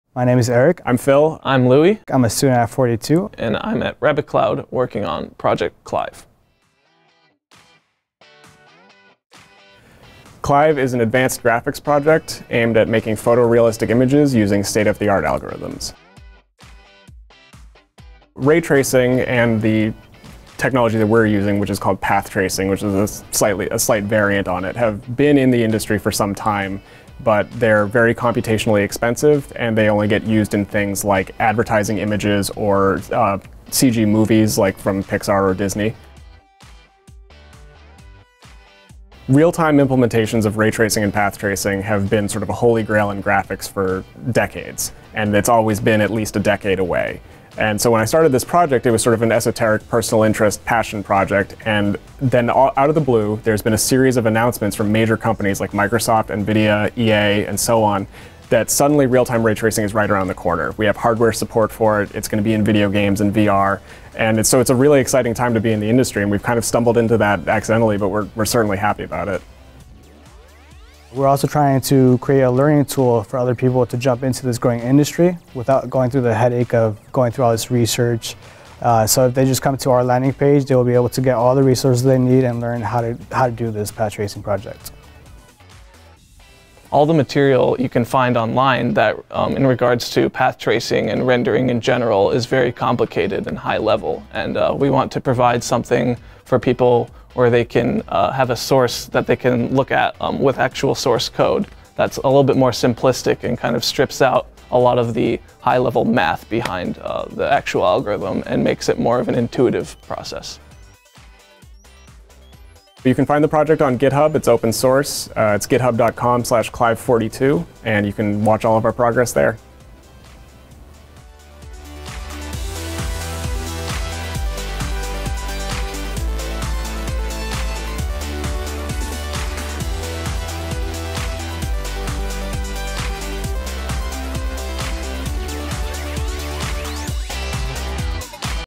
42 Accelerate: Interview of Clive
Our interview with team C.L.I.V.E., a student group working on a path tracing project.